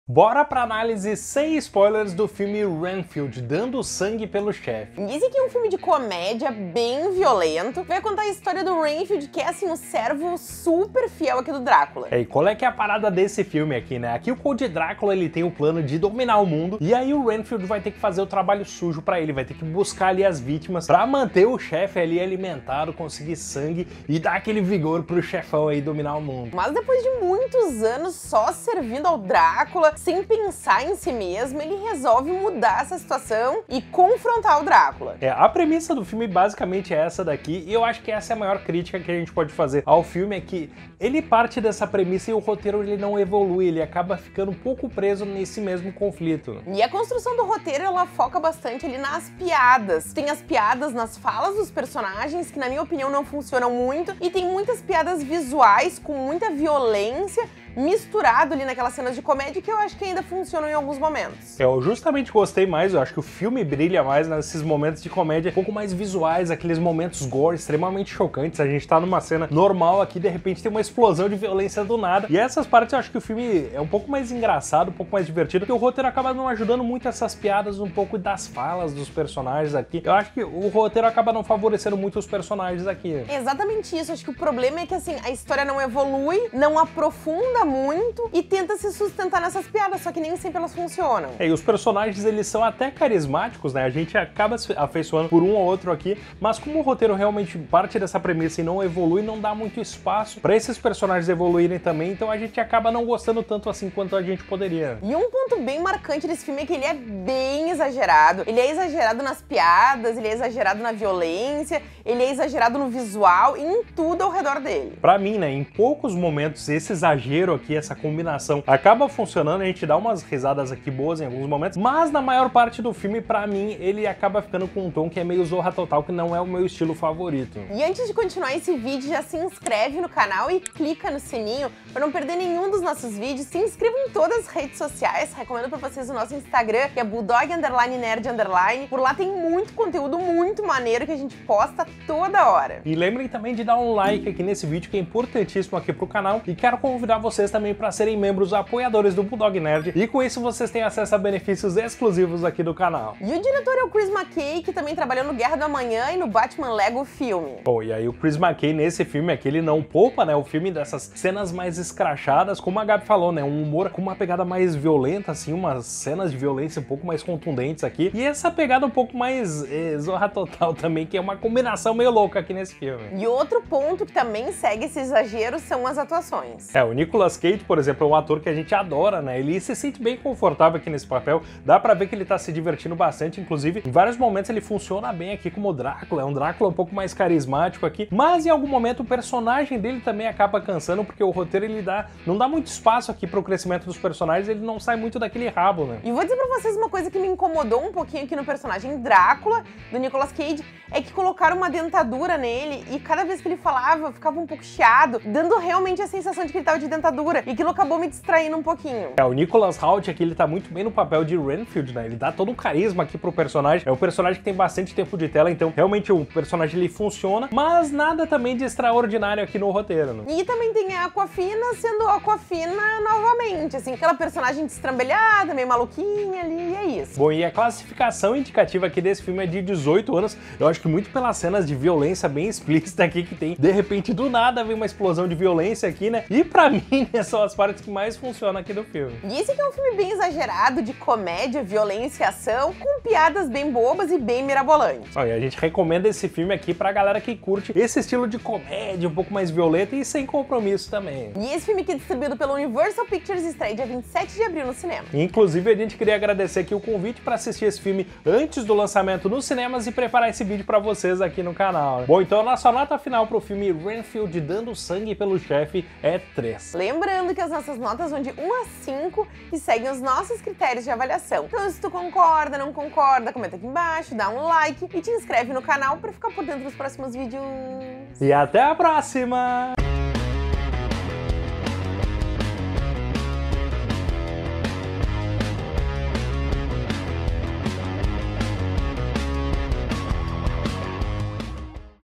RENFIELD: DANDO O SANGUE PELO CHEFE | Vale a pena assistir? | análise sem spoilers
Análise sem spoilers do filme "Renfield: Dando o Sangue Pelo Chefe".

⭐ID da PSN: bulldog_nerd_
⭐Gamertag do Xbox: BulldogNerdXbox
⭐Código de amigo Nintendo: SW-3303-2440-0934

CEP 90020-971
Porto Alegre-RS